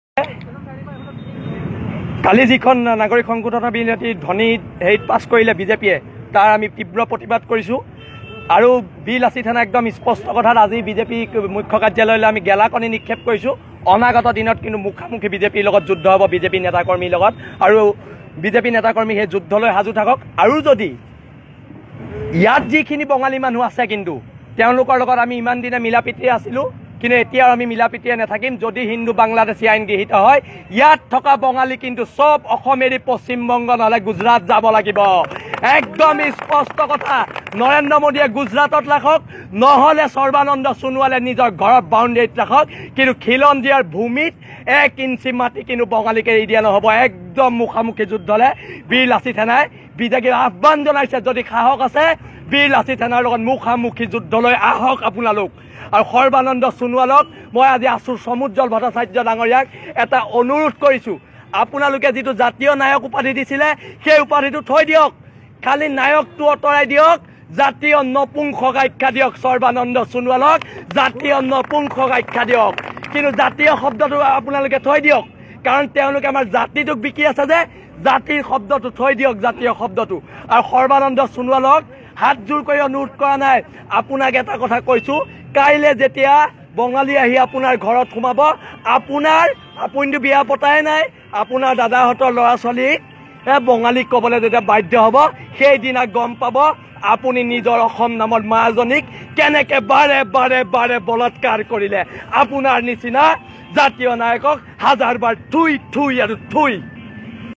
বিজেপি কাৰ্যালয় লৈ কোনে গেলা কণী নিক্ষেপ কৰিলে
অসম সেনাই কিয়নো এনে কৰিলে